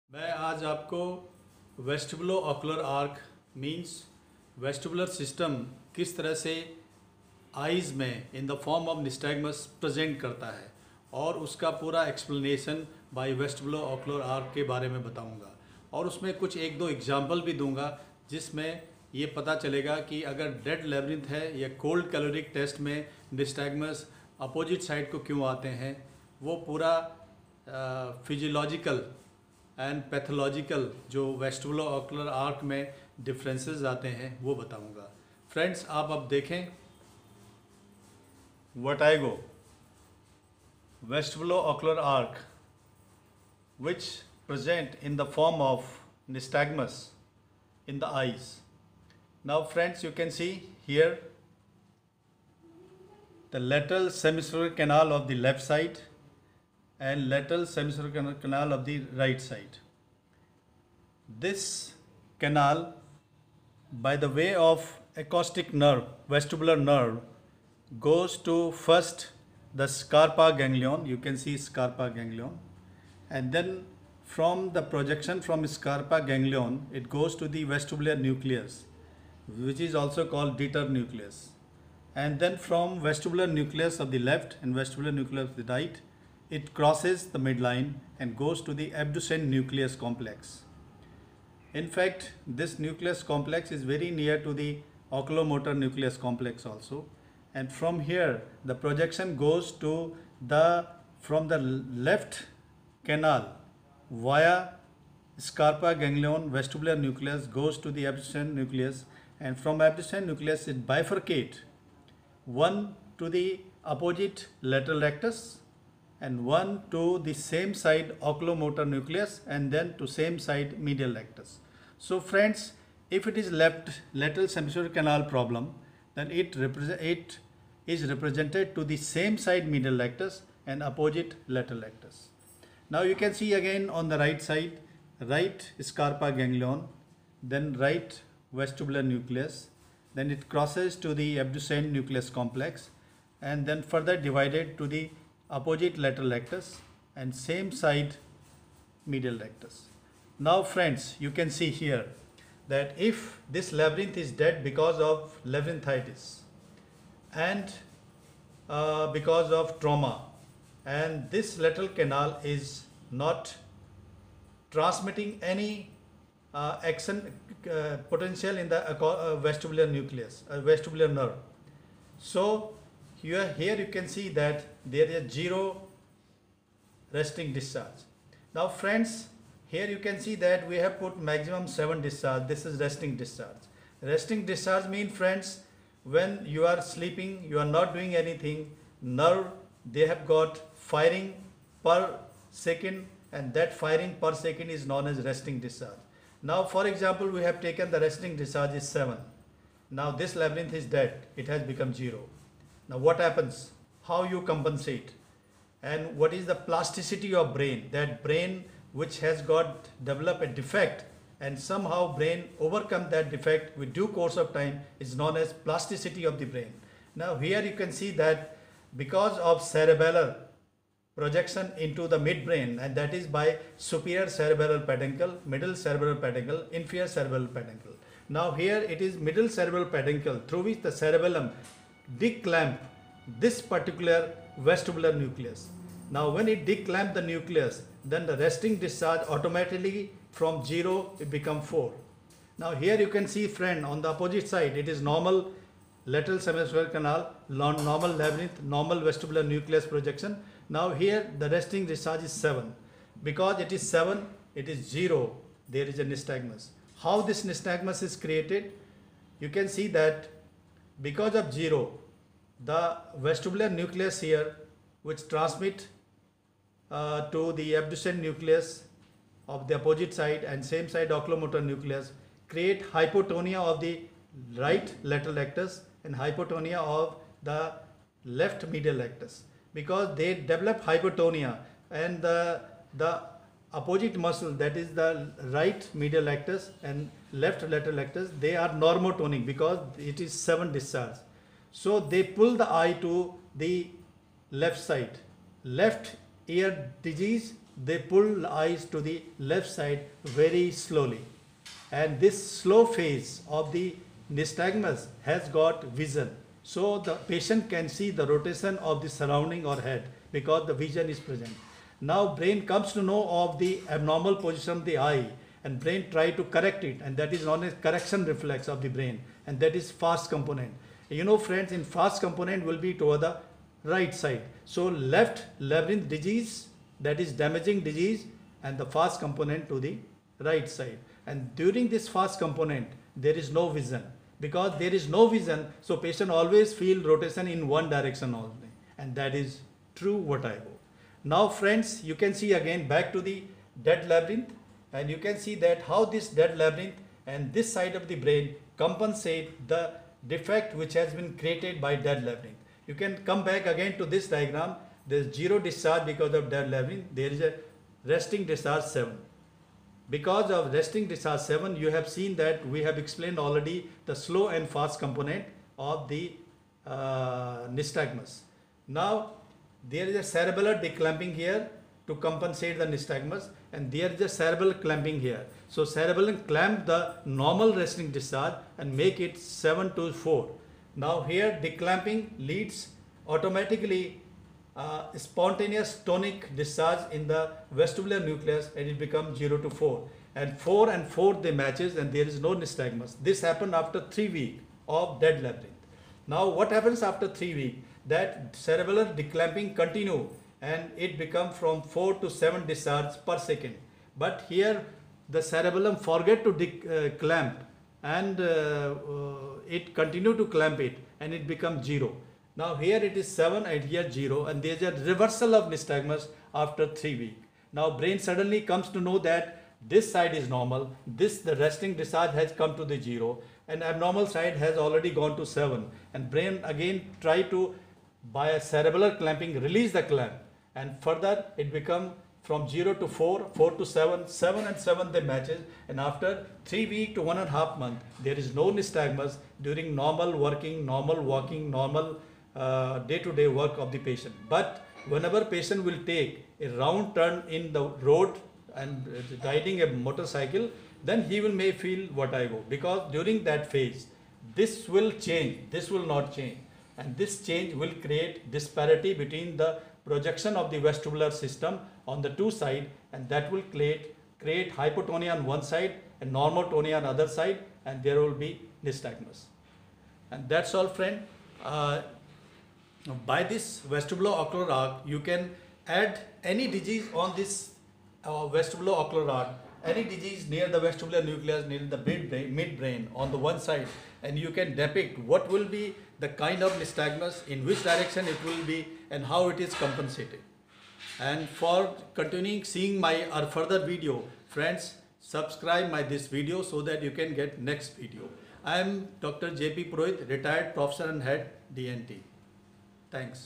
vestibulo ocular reflex explain vertigo.dr.j.p.purohit.retired prof.E.N.T.,dr.kartikeya purohit
vertigo explained by vestibulo-ocular reflex,.dr.j.p.purohit  and dr.kartikeya purohit M.S. E.N.T.